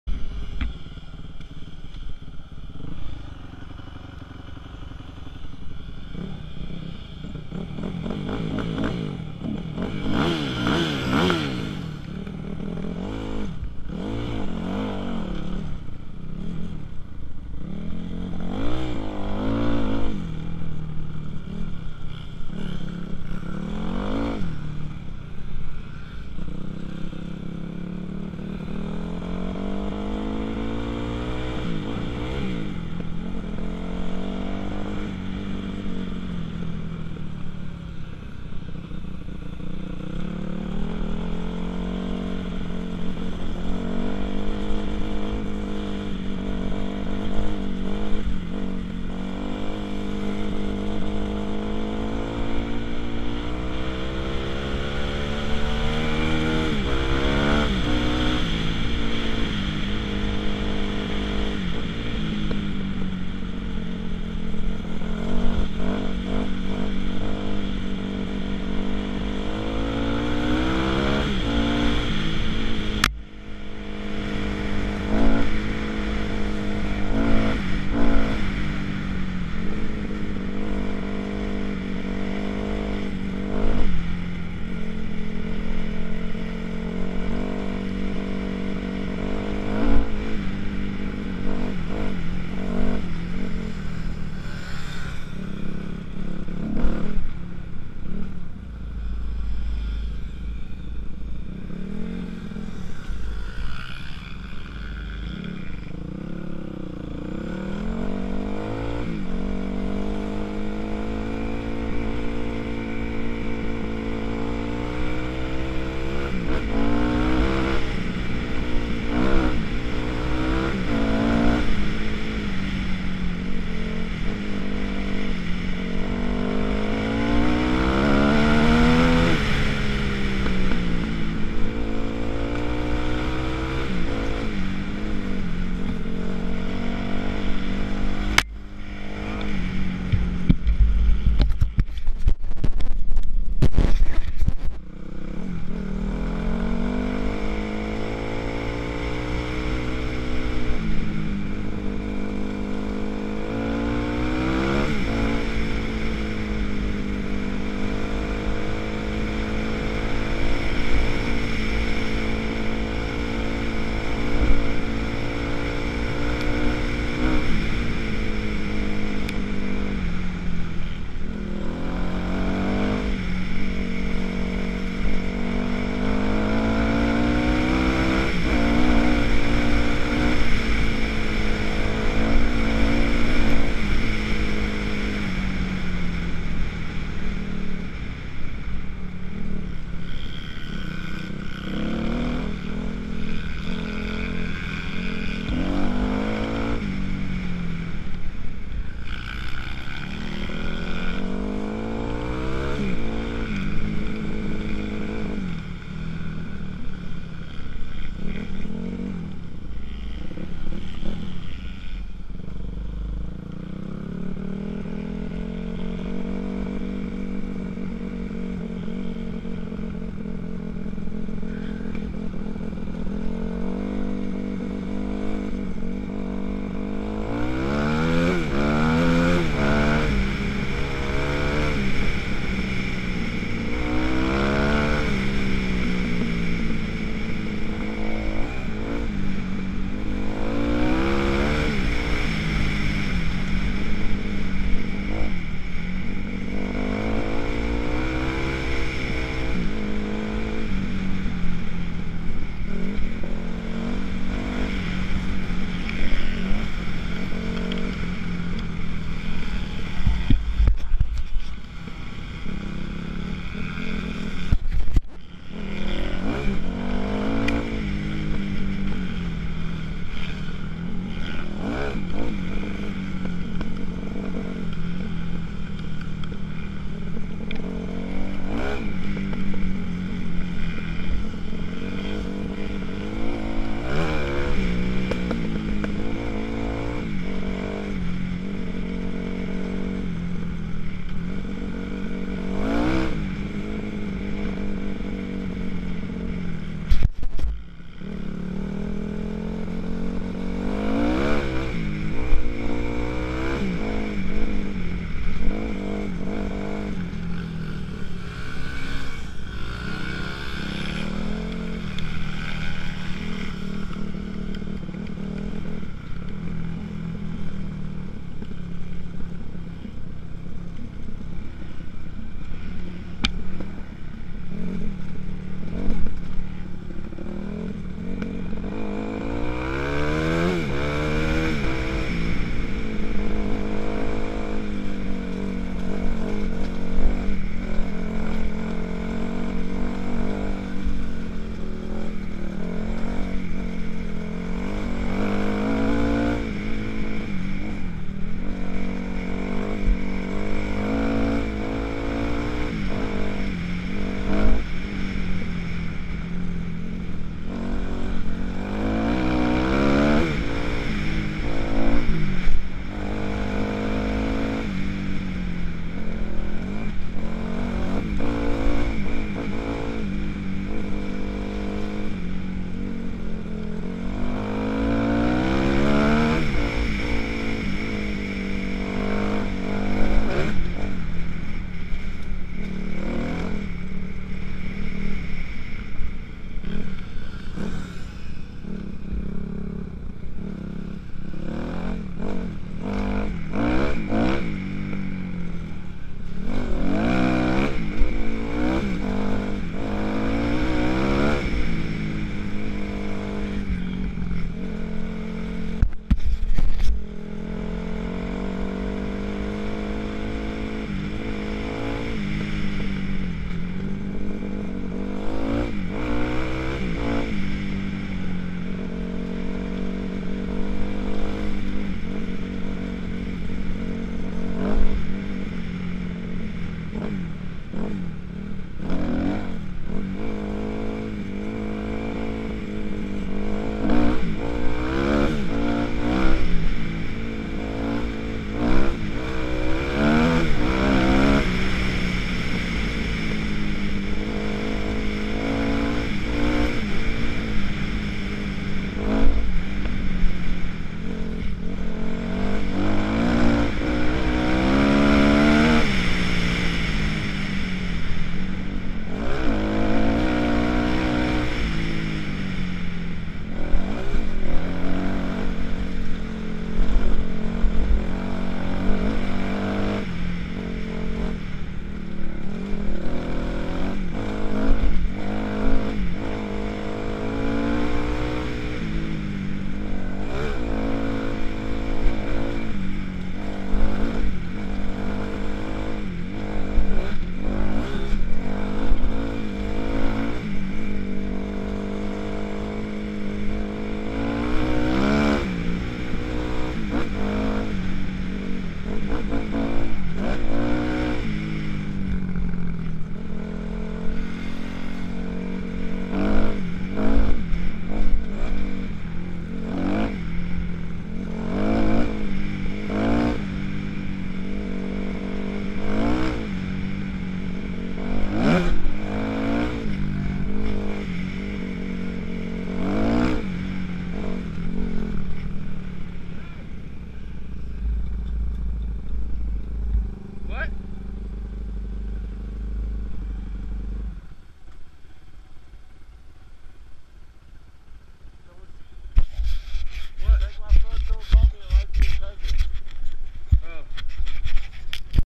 Kx250f trail riding in California
Up in the hills of Forrest hill California. Just some fun pov video of me on the trails. Riding a 2006 Kx250f 4 stroke.